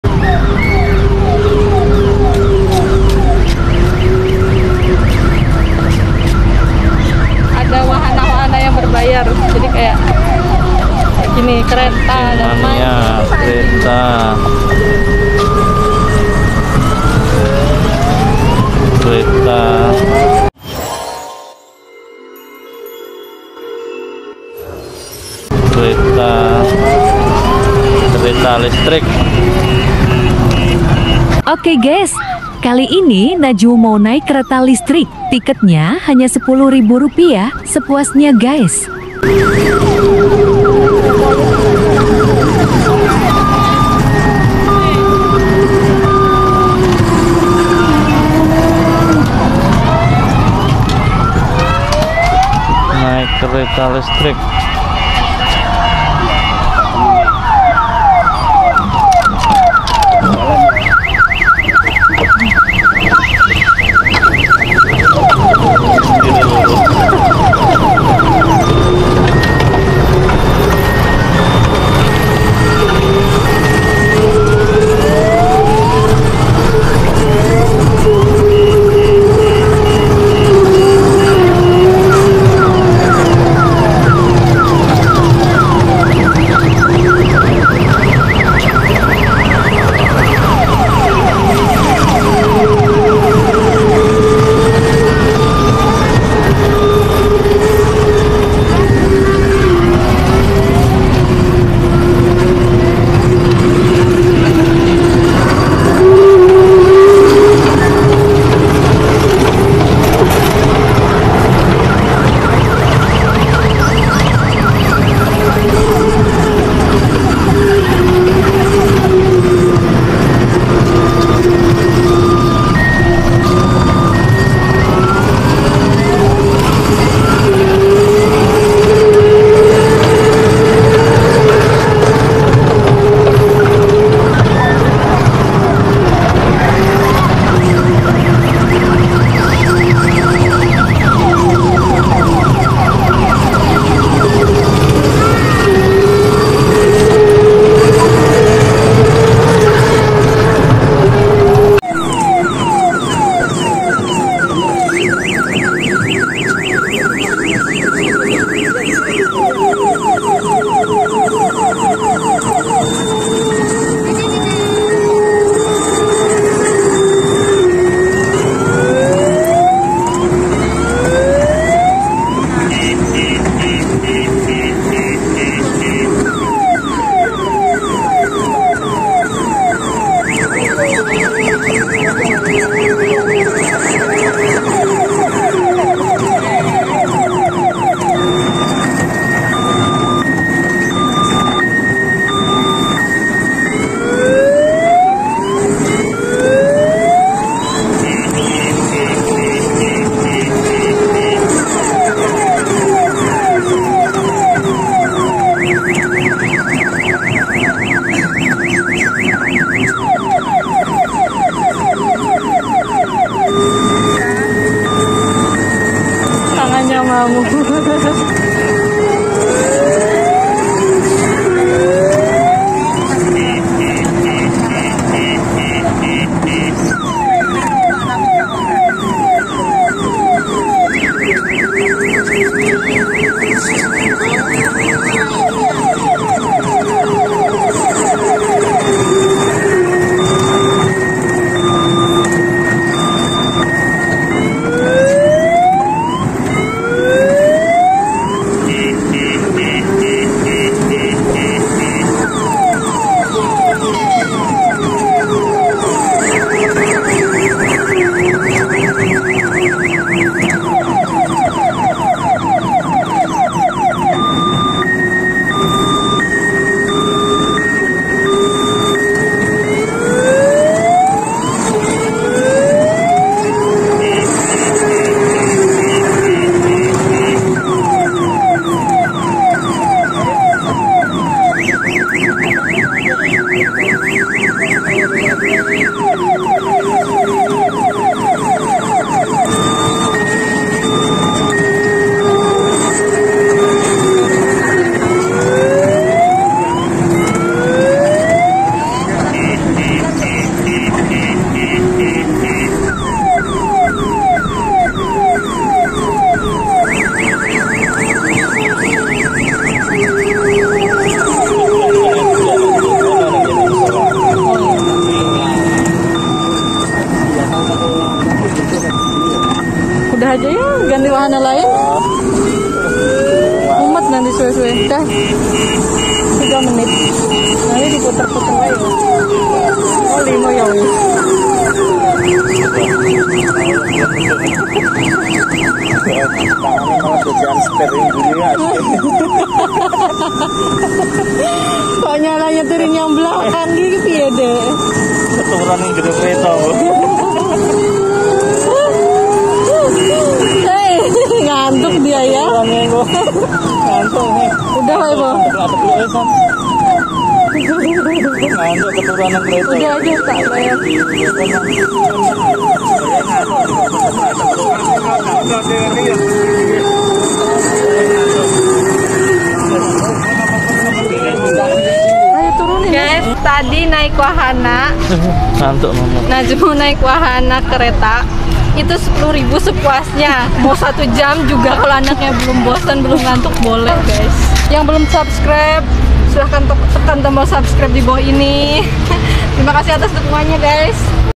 TAMAN BALEKAMBANG MANAHAN || NAIK MERETA API SEPULUH RIBU SEPUASNYA. KUAT BERAPA LAMA MAIN NIH?
di taman balekambang ada wahana atau arena bermain anak yang berbayar.
 salah satunya kereta listrik ini, kereta listrik ini tarifnya hanya 10.000 sepuasnya bermain